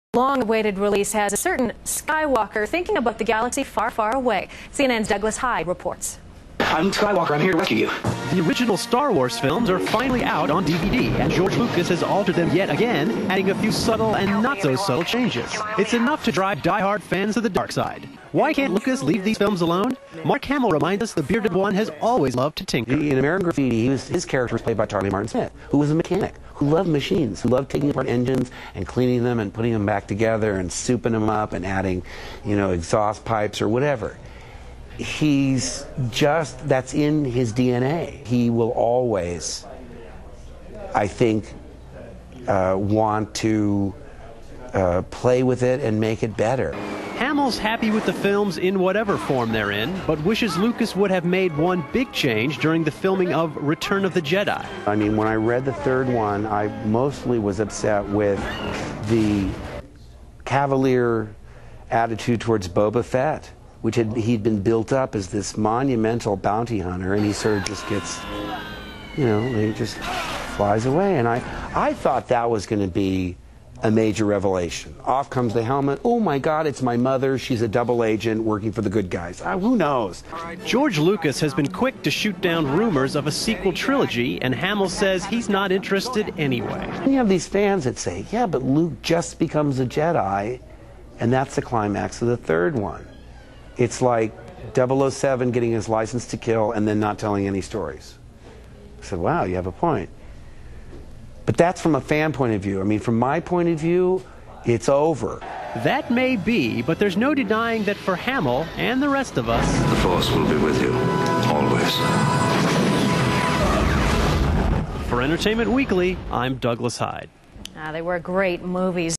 Mark Hamill on the DVD Releases
Interview Mark Hamill gave on the original trilogy being released to DVD